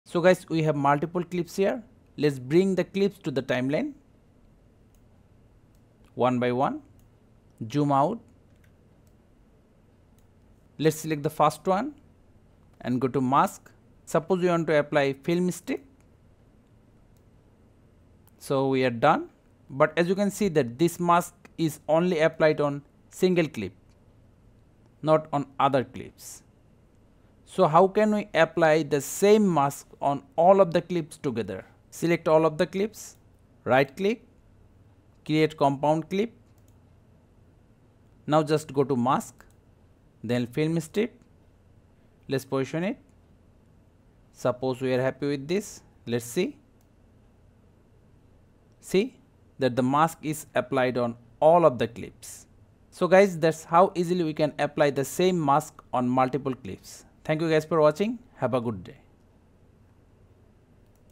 Apply Same Mask on Multiple Video Clips | CapCut PC Tutorial
How to apply mask on multiple video clips or layers at once in CapCut. This is a beginner friendly tutorial on CapCut Video Editing for Windows and Mac OS users.